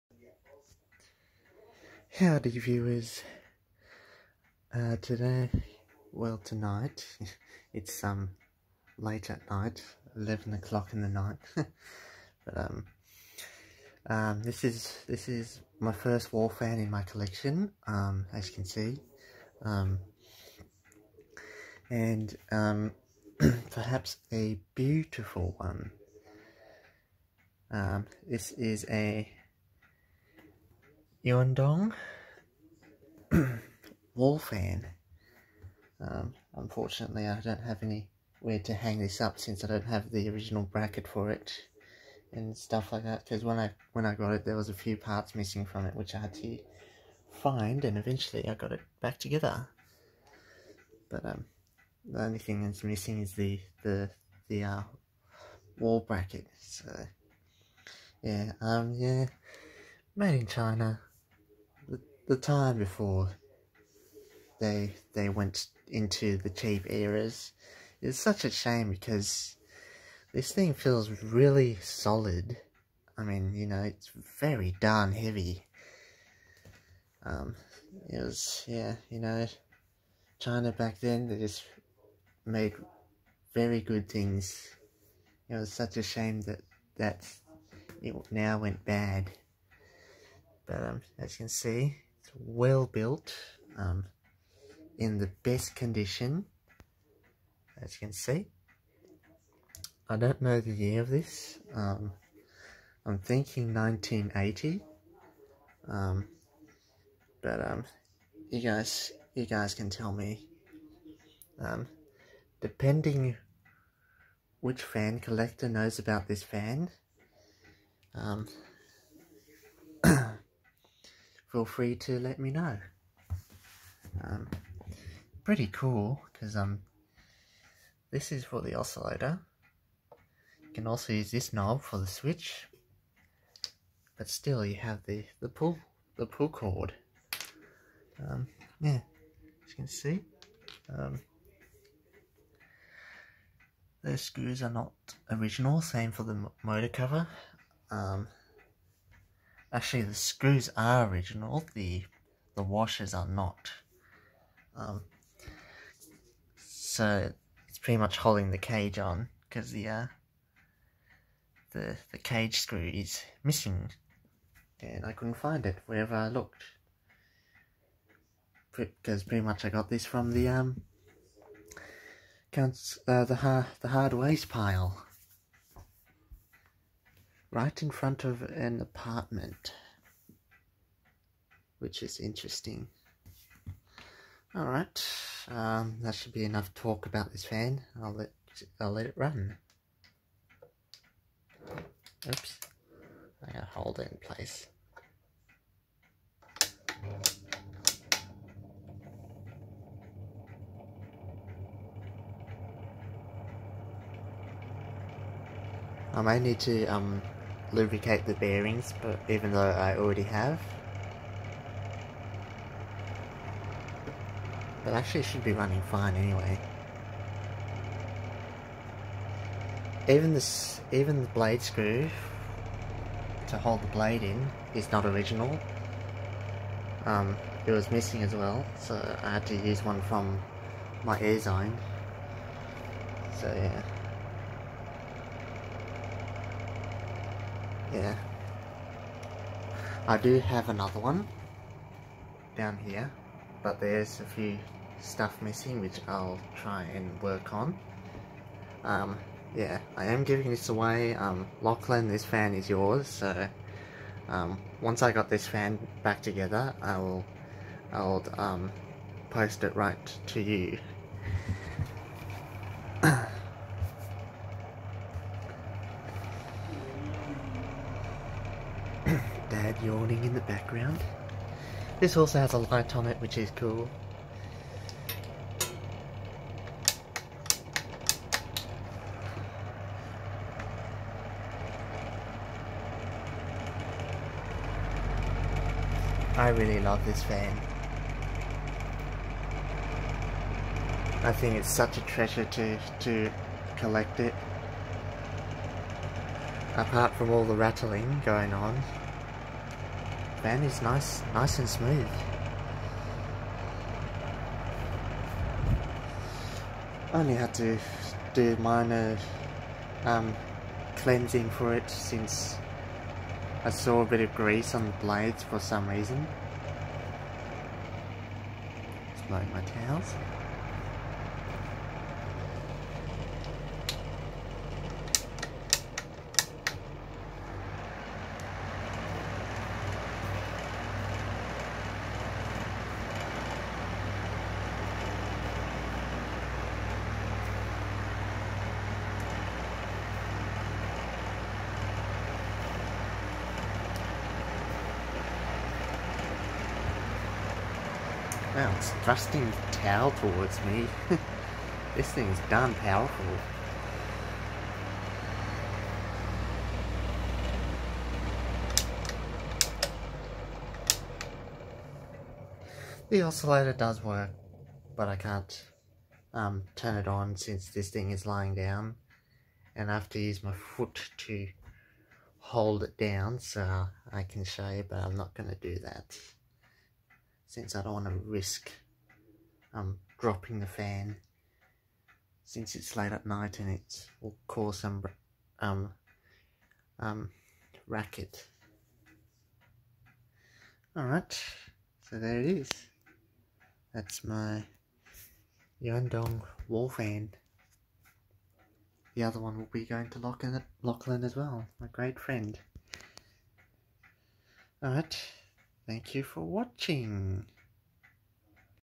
Yuan Dong Wall Fan
I never owned such masterpiece before from the great side of China, since I feel sorry for China that they were now told to make low value products. But overall, I still feel annoyed about their cheaply made products at the same time but some I trust. Not being racist or anything, just how I like certain items.

I do have another one of them, will be given away to Lachlan's Smash Channel and Fans.